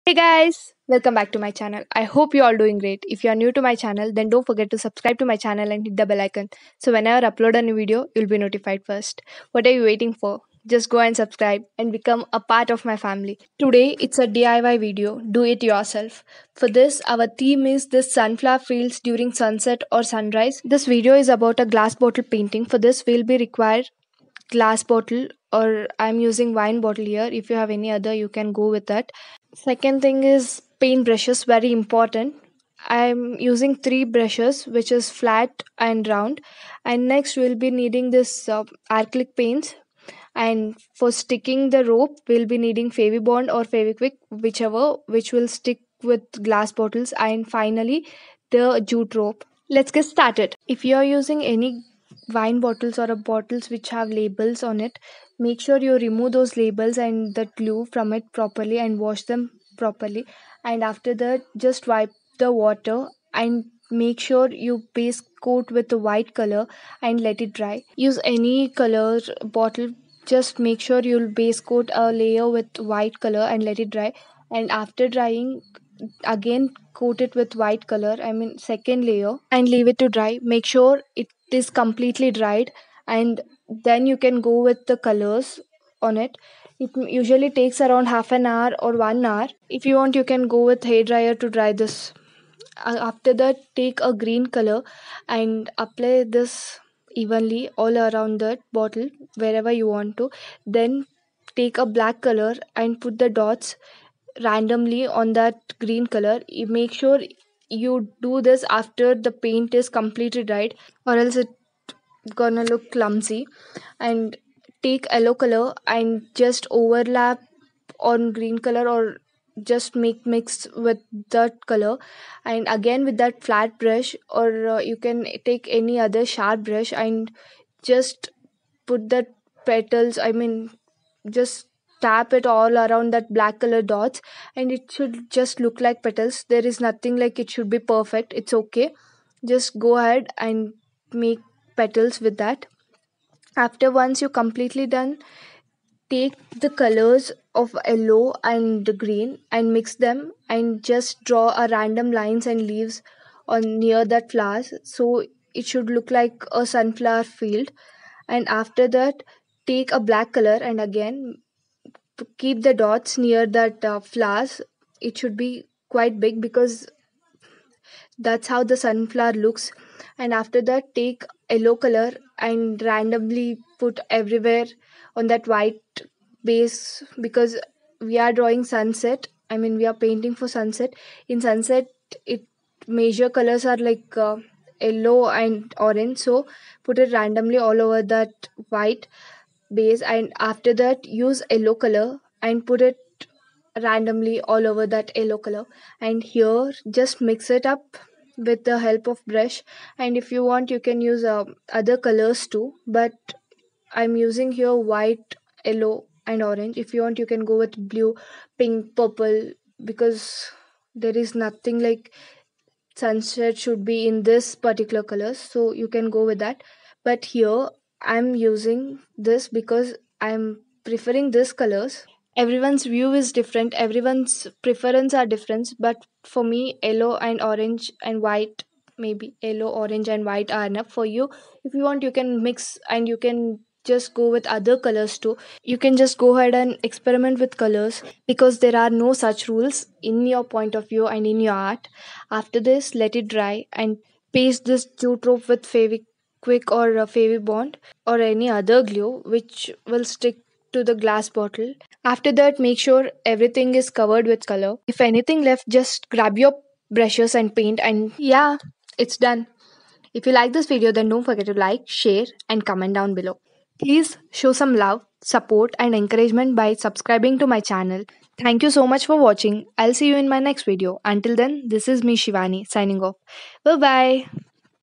How to paint sunflower field on Glass Bottle | Easy Glass Bottle painting | DIY | Arcylic Painting
Required materials:
1. Glass bottle
2. Paint brush
3. Arcylic paint
4. Fevibond or Fevikwik
5. Jute rope

Arcylic painting
Easy and quick DIY

love ❤
 Shivani Chowdary.